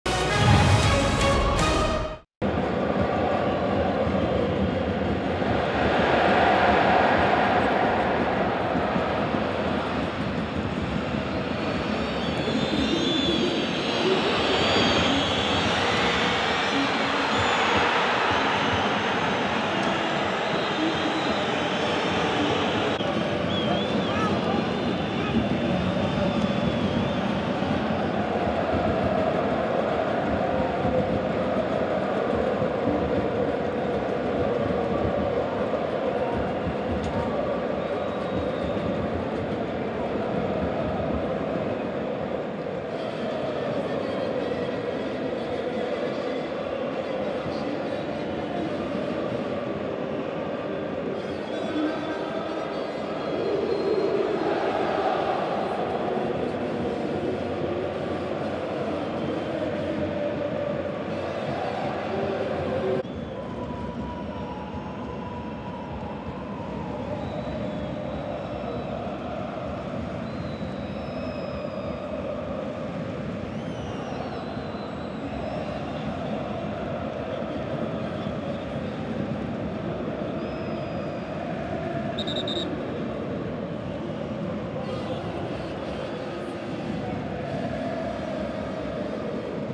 Goal Keita BALDE (4') / Olympique de Marseille - AS Monaco (2-2) / 2017-18
Olympique de Marseille vs AS Monaco (2 - 2) : Keita BALDE (4') goal. All Olympique de Marseille vs AS Monaco goals in video.

Orange Vélodrome - Sun, 28 Jan 2018

Goals : Keita BALDE (4' - AS Monaco) / Adil RAMI (7' - Olympique de Marseille) / Valère GERMAIN (47' - Olympique de Marseille) / FABINHO (51' - AS Monaco)